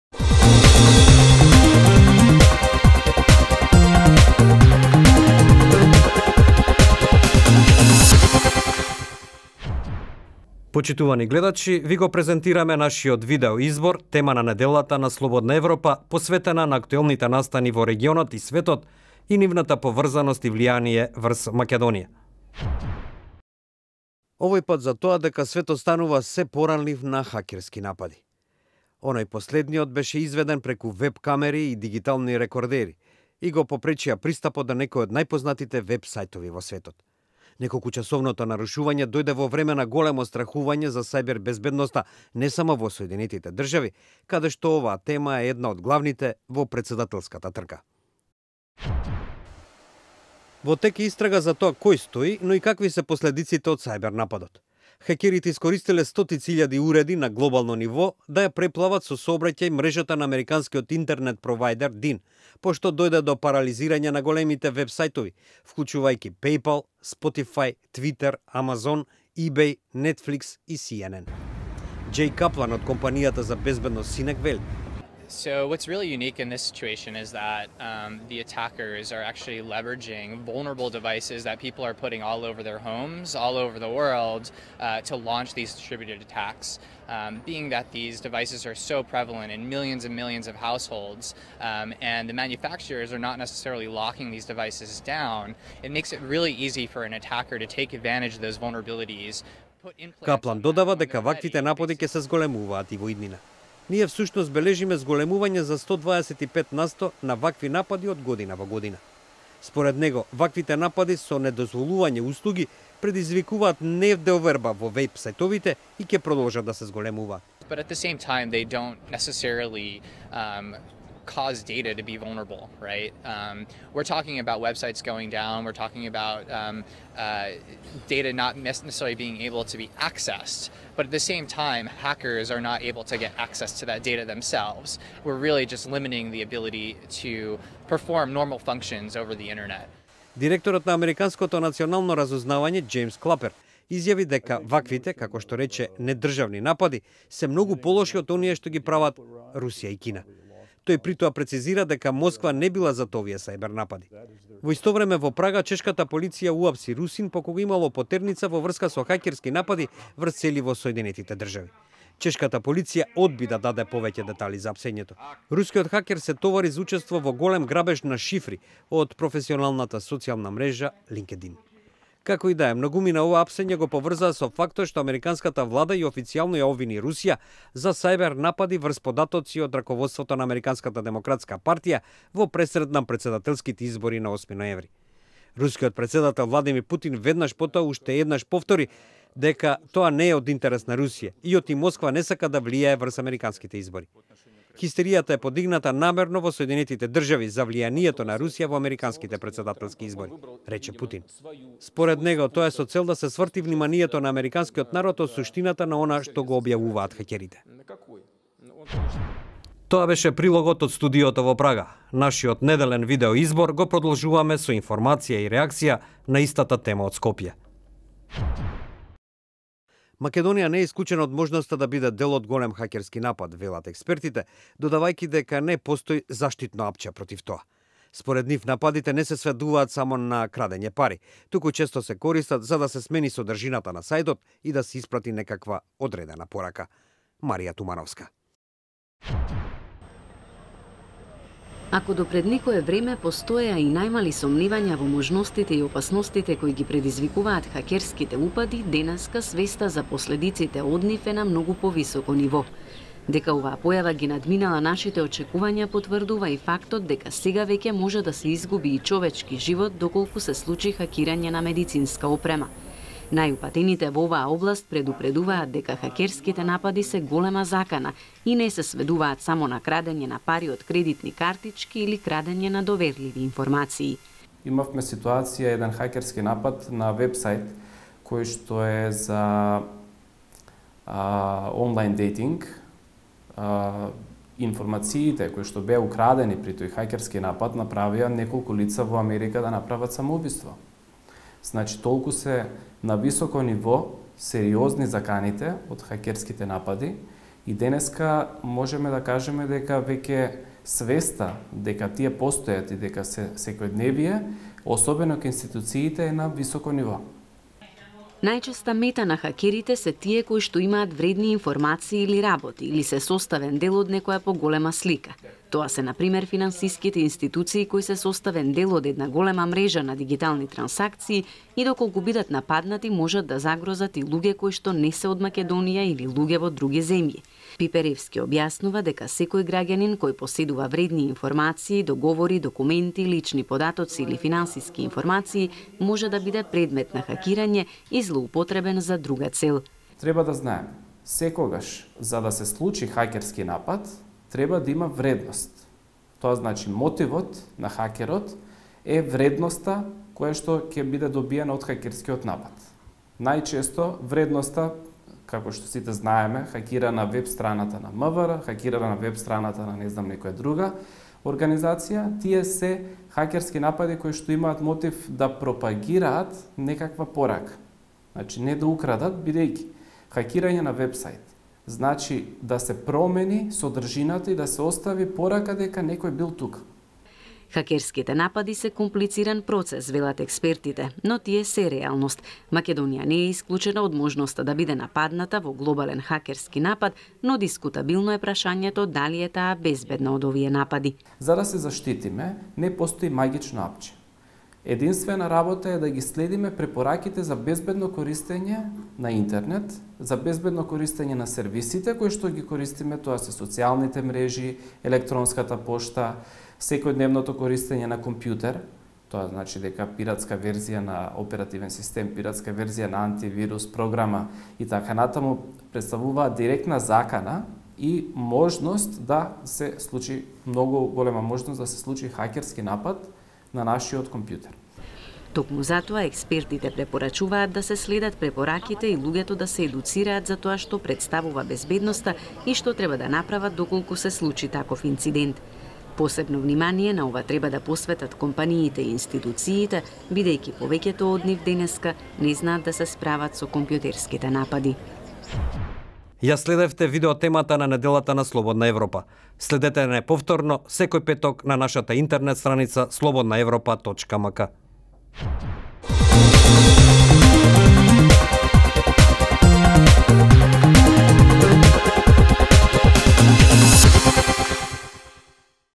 Светот станува се поранлив на хакерски напади
Хакерските напади уште еднаш го покренаа прашањето за тоа со каква опасност се соочуваат луѓето на интеренет. Страхувањето за сајбер безбедноста особено се поврзува со се поголемата поделеност меѓу Запад и Исток.